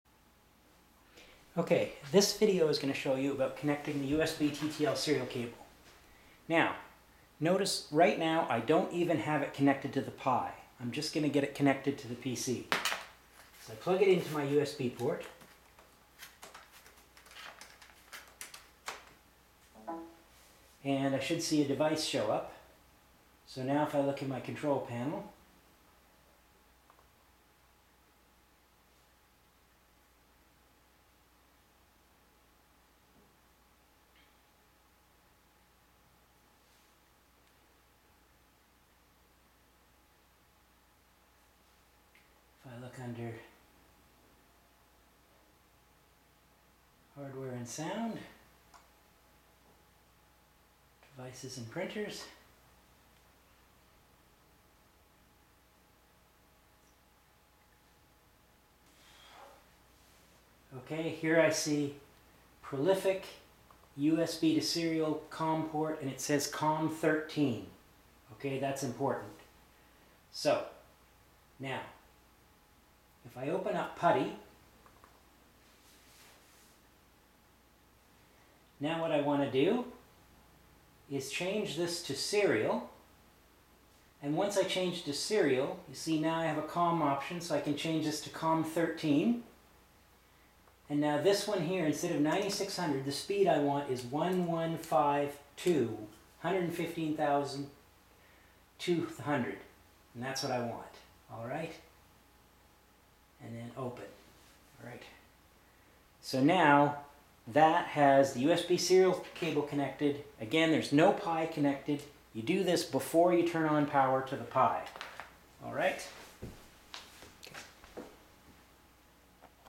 TTL-serial cable connection to PC
How to connect the TTL-serial cable to the PC and set up Putty for communication with Pi Zero